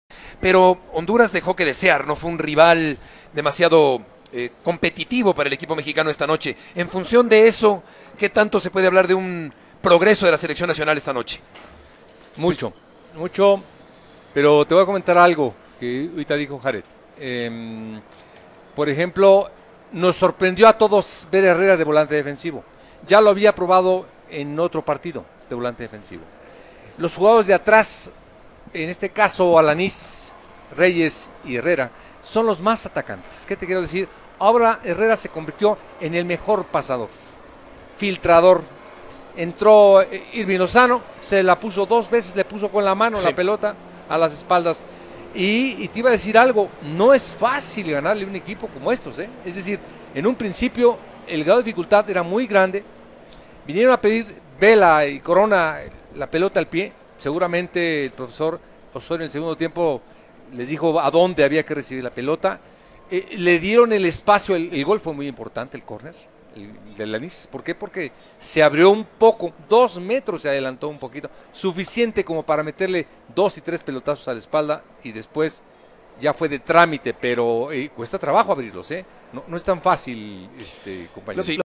Mario Carrillo: No es fácil ganarle a un equipo como Honduras
El Tri derrotó con claridad a la selección catracha, en duelo eliminatorio celebrado en el Estadio Azteca